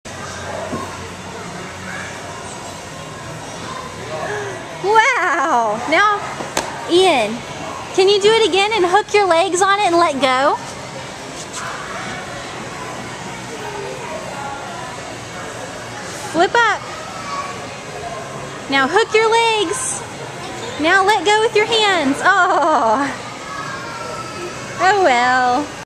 Acrobat Ian
Ian at Amerisport Gymnastics where I just taught him a trick.